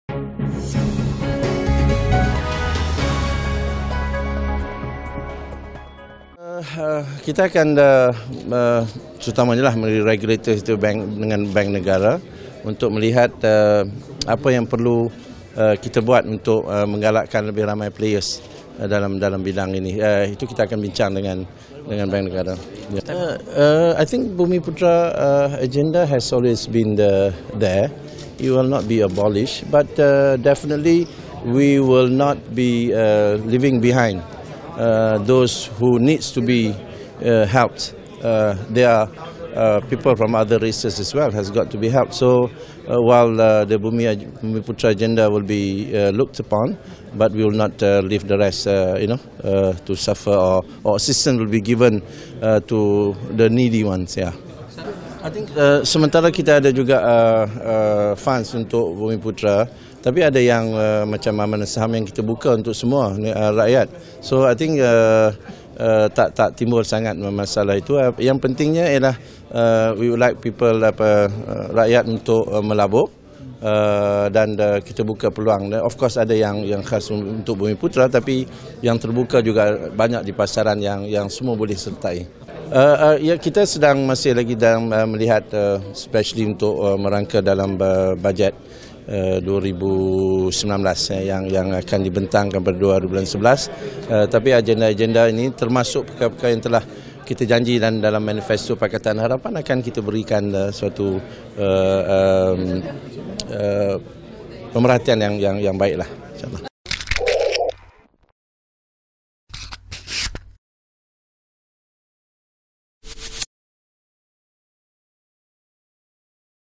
TERKINI : "Dasar Ekonomi Baharu (DEB) Diteliti Untuk Semua Kaum" -Timbalan Menteri Kewangan
Manifesto yang dibentangkan Pakatan Harapan sebelum memenangi pilihanraya umum ke-14 antara agenda utama yang bakal terkandung di dalam Bajet 2019.

Meskipun tidak menyatakan secara terperinci, Timbalan Menteri Kewangan, Datuk Ir Amiruddin Hamzah berkata, kementerian tersebut sedang meneliti mengenai Dasar Ekonomi Baharu (DEB).

Tegasnya, pendirian Pakatan Harapan amat jelas di dalam mengekalkan agenda orang Melayu dan bumiputera namun dalam masa yang sama percaya bahawa kaum lain juga perlu mendapat perhatian kerajaan.

"Agenda Bumiputera memang sentiasa dipegang oleh Pakatan Harapan tetapi kami juga mahu kaum lain tidak ditinggalkan.

"Kami konsisten di dalam memberi bantuan kepada semua rakyat yang memerlukan tanpa mengira kaum," katanya kepada pemberita selepas merasmikan Persidangan dan Pameran Tahunan BankTech Asia ke-10 di sini hari ini.